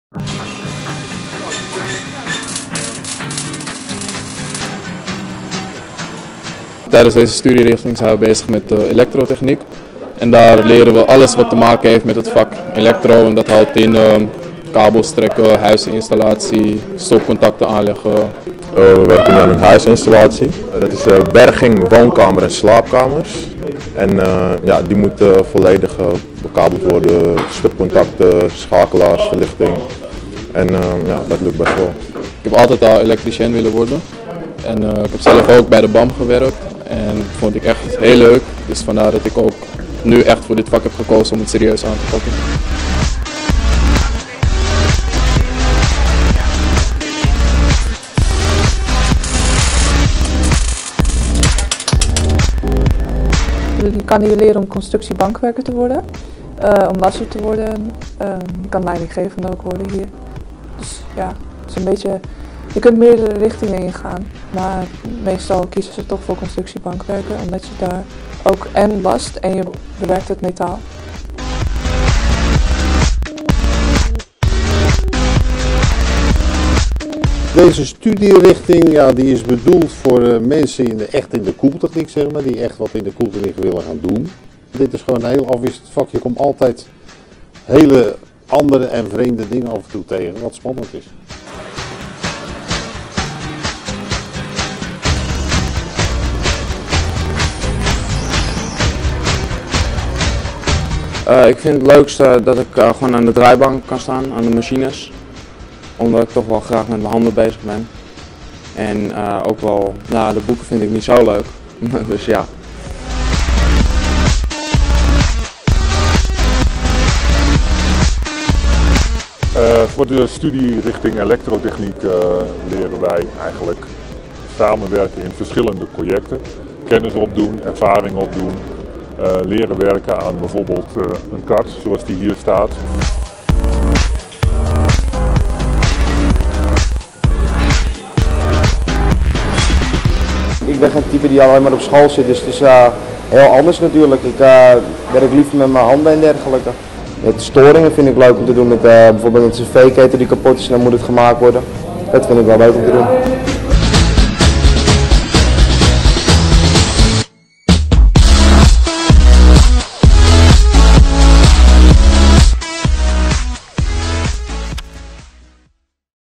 Techniek & procesindustrie opleidingen
Ontwerpen van producten, installeren van een airco, leidinggeven, het maken van berekeningen, inspecteren van systemen, bouwen en monteren. Techniek en Procesindustrie is breed, interessant en afwisselend.